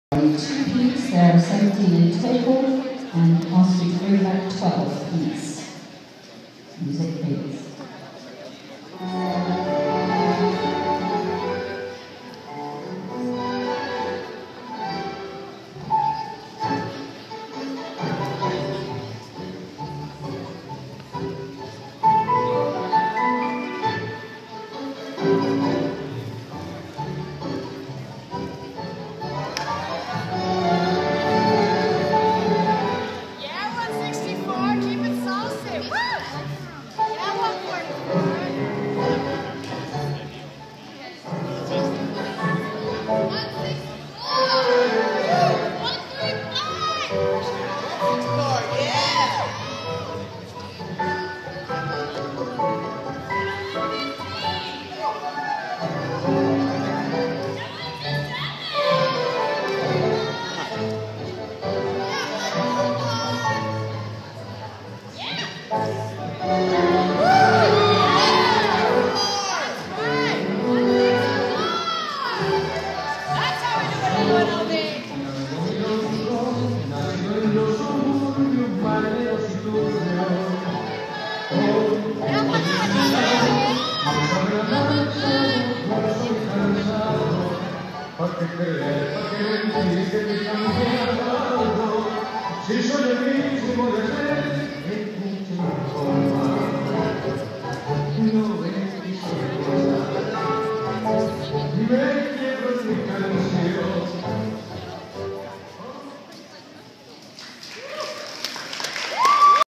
David and Whitney dancing the Argentine Tango
Desert Challenge Dancesport Championships.  Our first competition together as husband and wife, David's very first dance comp, and my first time to compete in Argentine Tango.  We had a wonderful time and in this dance we made it to the quarter finals. -3